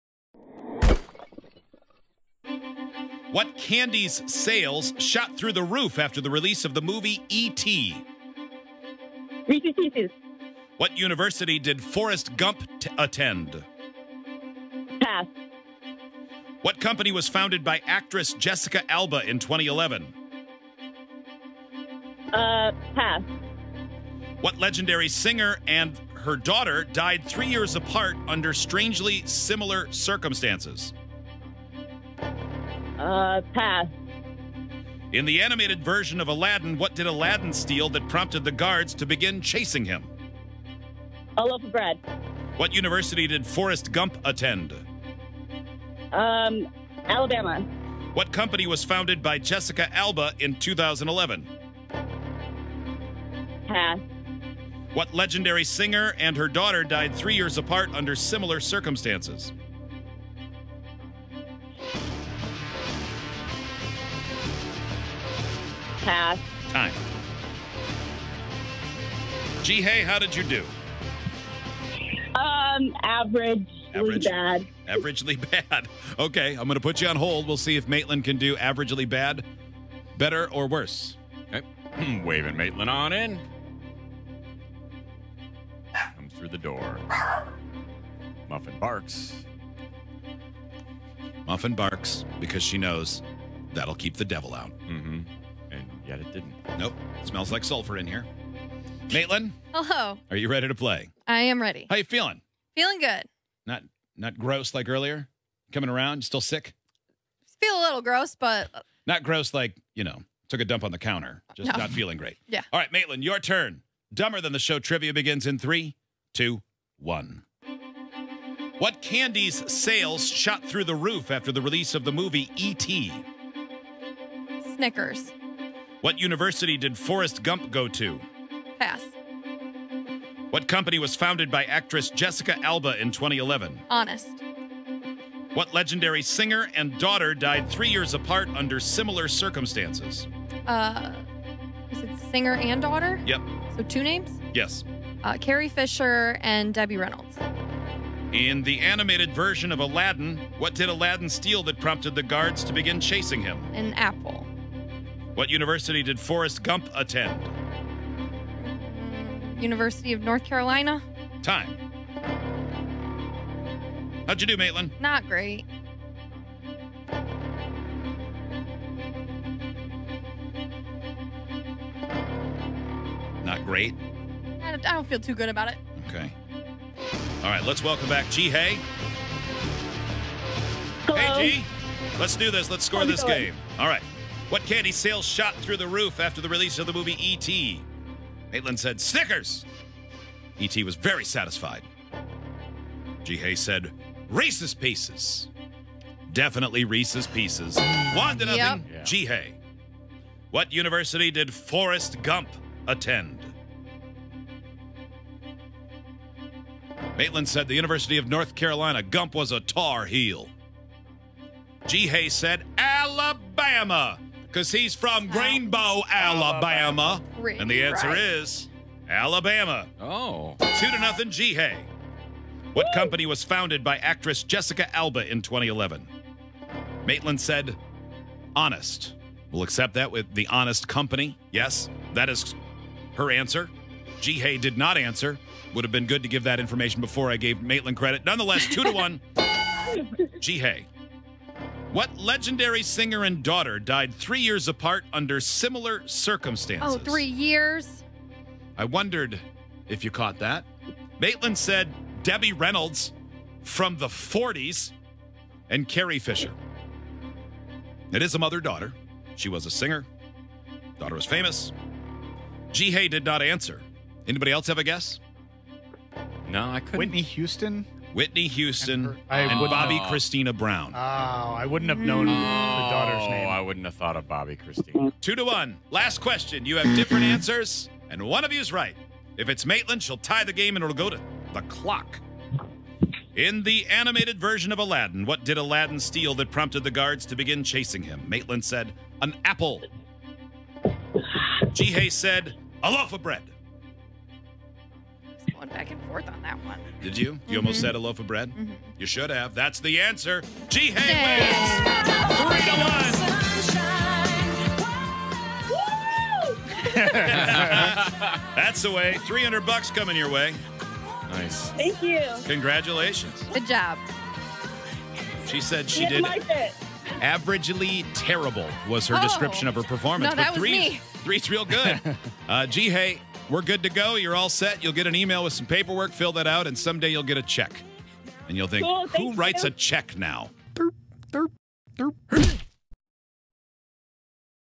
What University Did Forrest Gump Attend?
Each morning on the show, we play Dumber Than The Show Trivia for a cash prize. Today, Jihye in Grand Rapids, MI took on Maitlynn on a $300 round of the game. Watch the clip below to find out if the pot increases, or if it resets to $100!